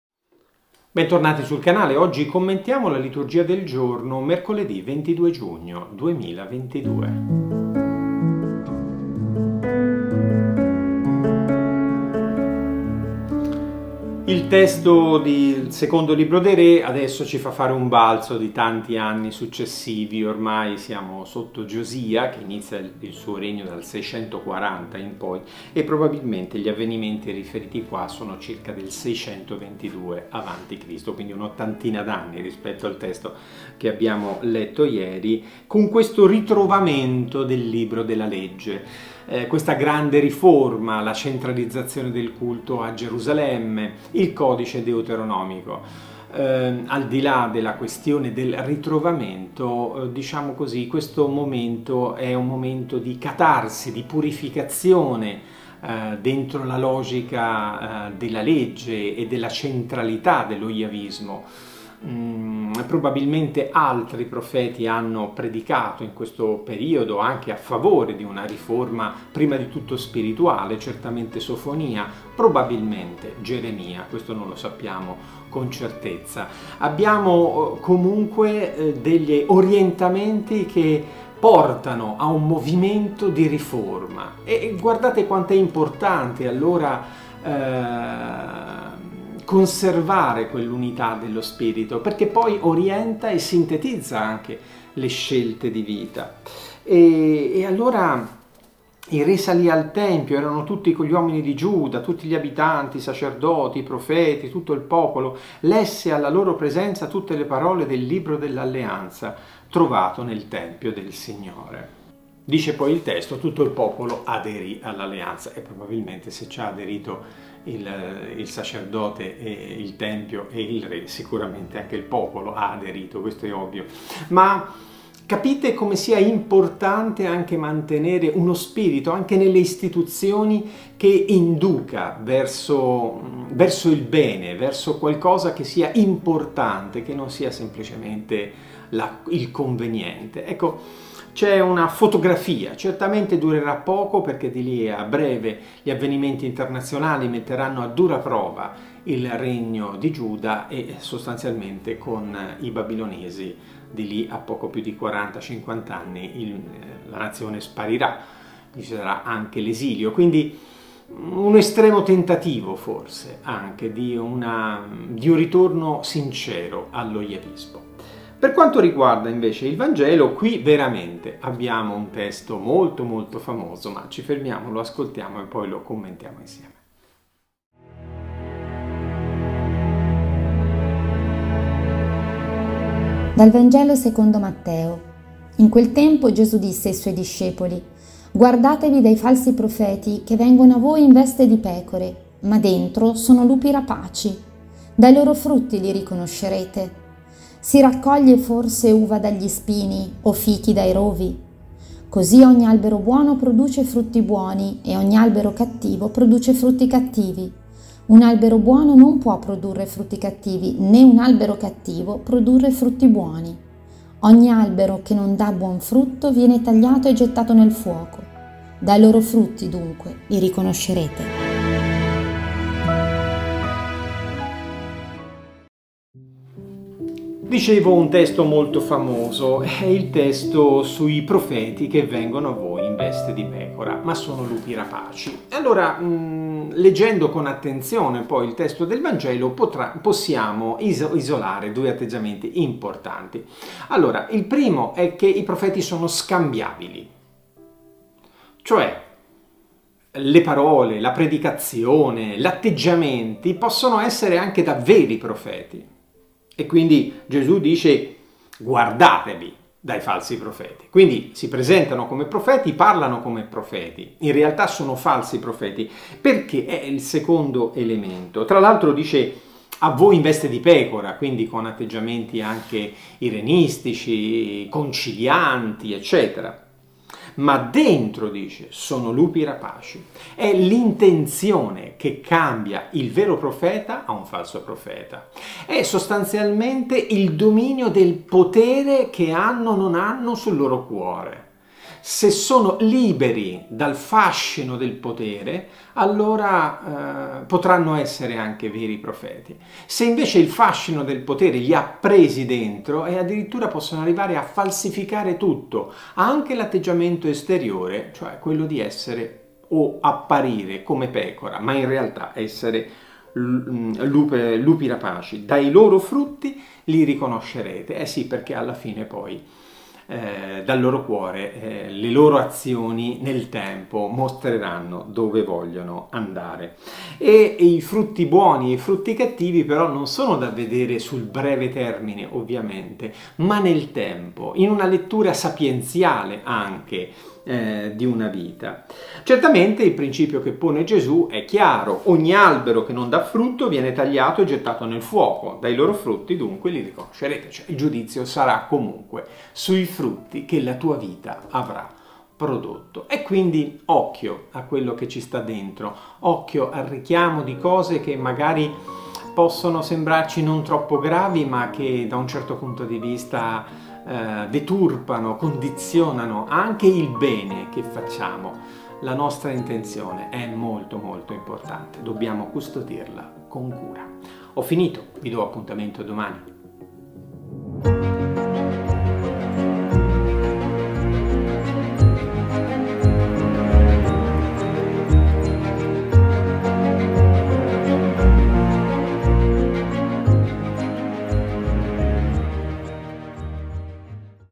Liturgia del giorno (22 giugno 2022)
del giorno 22 GIUGNO 2022

2Re 22,8-13; 23,1-3; Sal 118(119);
Mt 7,15-20: "Guardatevi dai falsi profeti che vengono a voi in veste di pecore, ma dentro sono lupi rapaci! Dai loro frutti li riconoscerete".